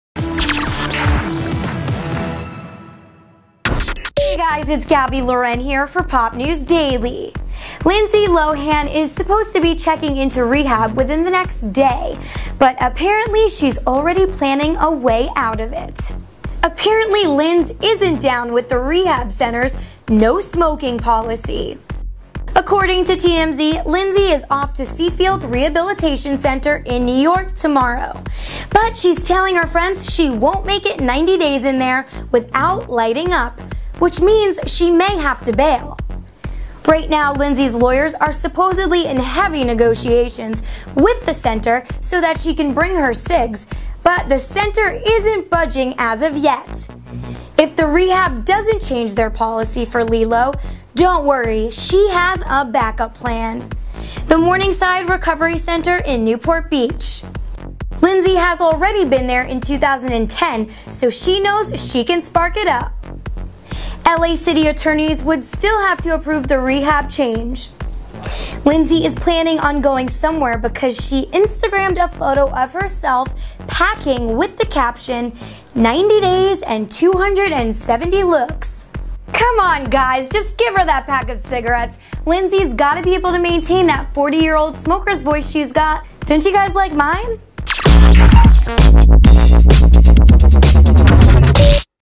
Lindsay Lohan Can't Smoke In Rehab
Lindsay Lohan may not be allowed to smoke in rehab... Uh-oh.